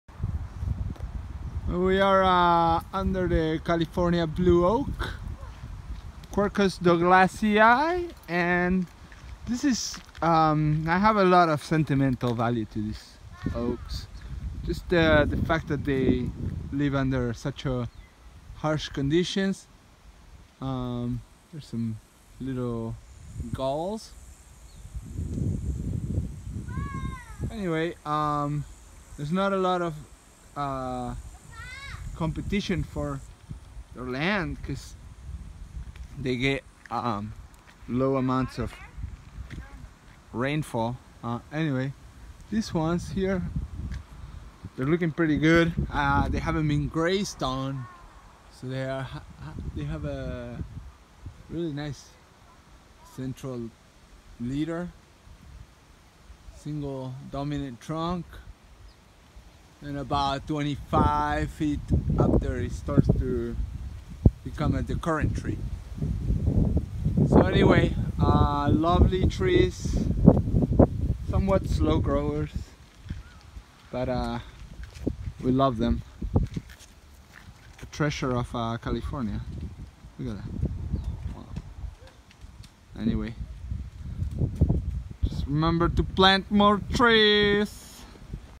California Blue Oak
Quercus douglasii, known as blue oak, is a species of oak endemic to California, common in the Coast Ranges and the foothills of the Sierra Nevada. It is California's most drought-tolerant deciduous oak, and is a dominant species in the blue oak woodland ecosystem